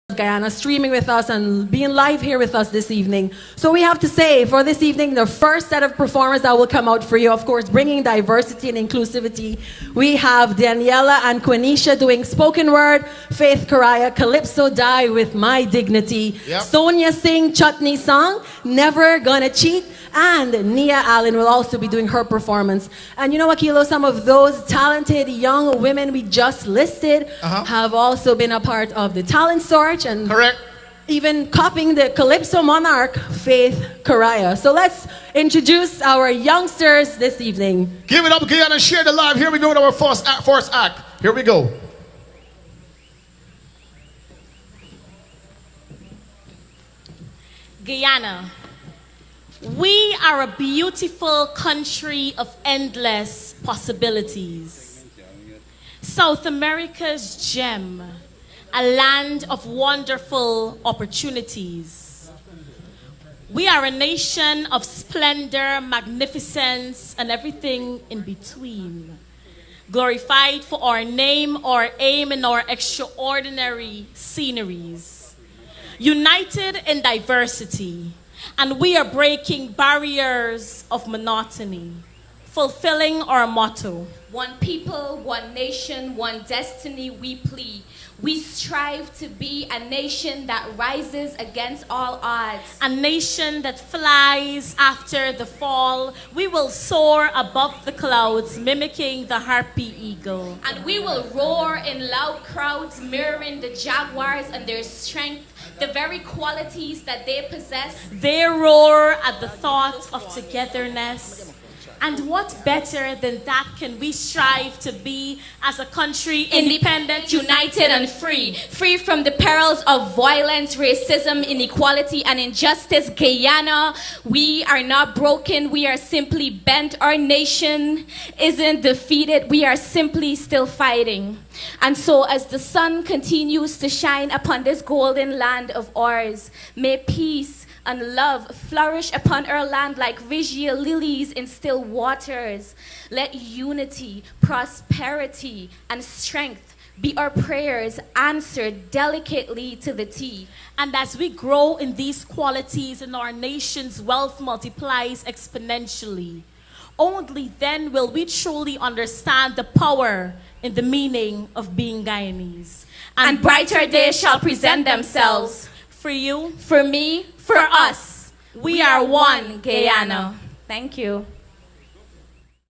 Daniella and Quanisha @ One Guyana concert
Daniella and Quanisha who are talented citizens of guyana makes an appearance at the One Guyana concert.
A platform to showcase the beauty of Guyana expressed through its diverse culture, people and natural scenery.